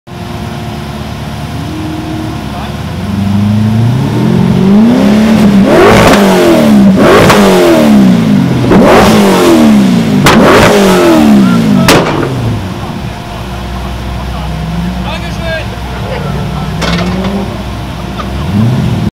Carporn.at - FastCarFestival Soundcheck #4 EXTREM
Auch die Drifter in der Lausitz waren nicht von schlechten Eltern! Kopfhörer auf und genießen!